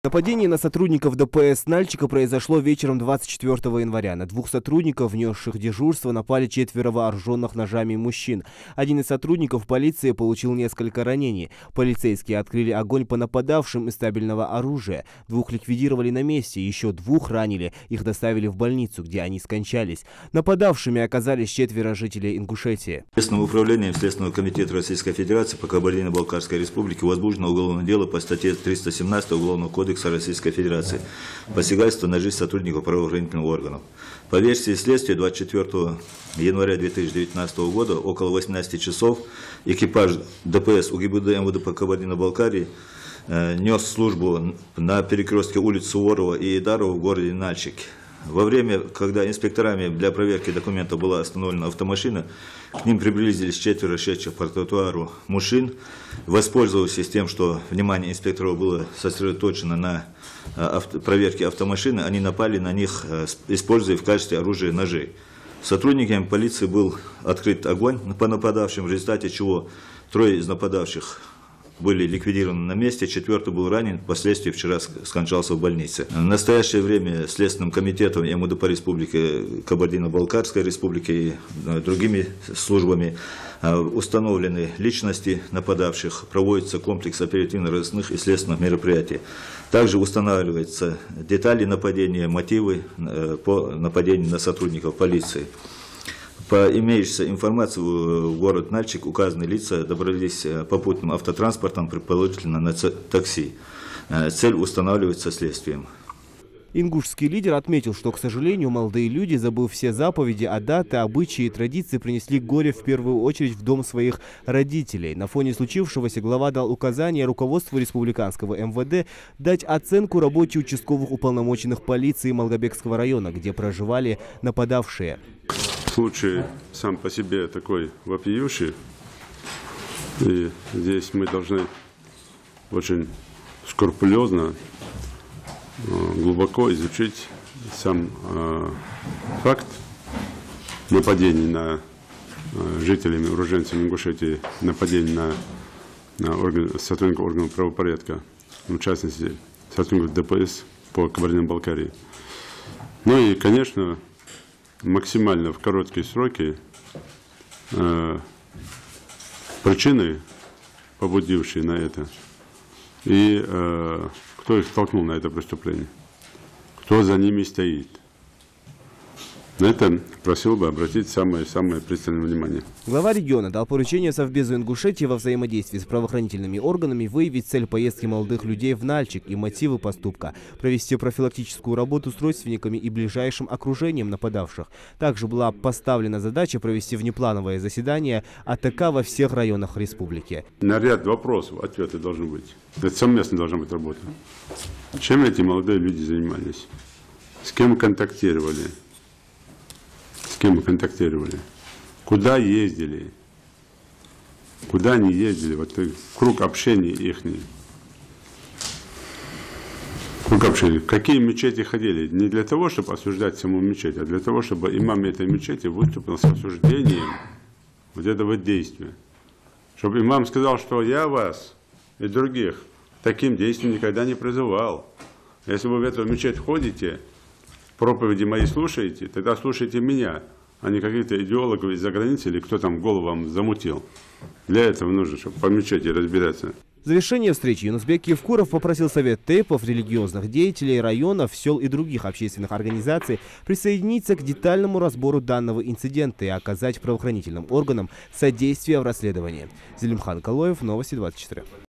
Евкуров потребовал установить сообщников молодых людей, напавших на экипаж ДПС в Нальчике.
Глава Ингушетии Юнус-Бек Евкуров в ходе внепланового заседания антитеррористической комиссии поручил провести оперативные и  профилактические мероприятия по установлению и задержанию лиц, которые могли подтолкнуть жителей республики к совершению нападения на экипаж ДПС в Кабардино-Балкарии. "Случай сам по себе вопиющий. Мы должны скрупулёзно, глубоко изучить сам факт нападения жителей нашей республики на сотрудников органов правопорядка КБР. Конечно, в максимально короткие сроки надо установить причины, побудившие их на это, кто их толкнул на это преступление, кто за ними стоит", - сказал Ю. Евкуров.Ингушский лидер отметил, что, к сожалению, молодые люди, забыв все заповеди, адаты, обычаи и традиции, принесли горе в первую очередь в дом своих родителей.«Этот случай еще раз доказывает одно - борьбу с такой болезнью современности, как экстремизм, нельзя прекращать. Вопрос о реализации профилактических мер, направленных на недопущение вовлечения молодежи в террористическую и экстремистскую деятельность, остается актуальным», - отметил Глава региона.Глава региона дал поручение Совбезу Ингушетии во взаимодействии с правоохранительными органами выявить цель поездки молодых людей в Нальчик и мотивы поступка, провести профилактическую работу с родственниками и ближайшим окружением нападавших. Также была поставлена задача провести внеплановые заседания АТК во всех районах республики. Уполномоченному по правам человека в РИ поручено выехать в Кабардино-Балкарию для содействия расследованию и контакта с семьей пострадавшего.

В завершение встречи Ю. Евкуров попросил Совет тейпов, религиозных деятелей районов, сел и других общественных организаций присоединиться к детальному разбору данного инцидента и оказать правоохранительным органам содействие в расследовании.Напомним, вечером 24 января в Нальчике на экипаж ДПС, остановивший автомашину для проверки документов, напали четверо вооруженных ножами мужчин. Один из сотрудников полиции получил несколько ранений. Полицейские открыли огонь по нападавшим из табельного оружия. Двух ликвидировали на месте, еще двух ранили. Их доставили в больницу, где они скончались. Нападавшими оказались четверо жителей Ингушетии.

Следственное управление СК РФ по КБР возбудило уголовное дело о посягательстве на жизнь сотрудника правоохранительных органов (ст. 317 УК РФ).